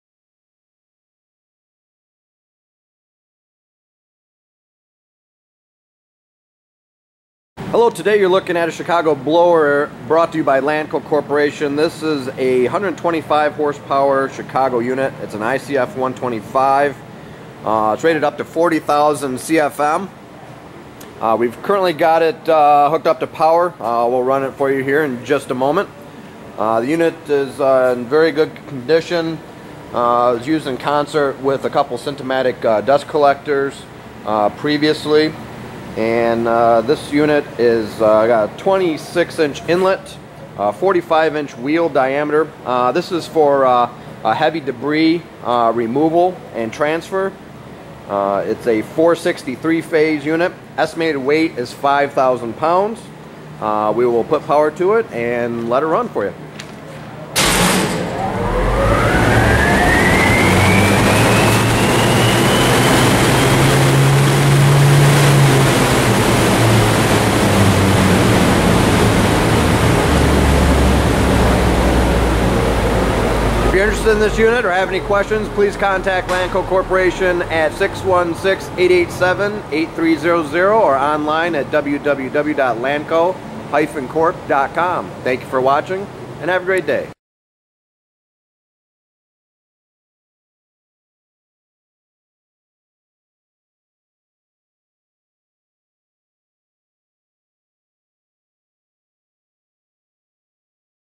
Chicago Blower 40,000 CFM 125 HP Steel Exhaust Blower EB2055
Manufacturer       Chicago Blower
Model #                ICF125
Material                Steel
CFM                     40000
HP                        125
Volts                     460
Inlet (inches)         26
Tested By             DB
OAD                     108"L x 90"W x 90"H
Phase                   3
Weight                  5000

ADDITIONAL INFORMATION
 45" wheel diameter.

Lanco LLC
665 10 Mile NW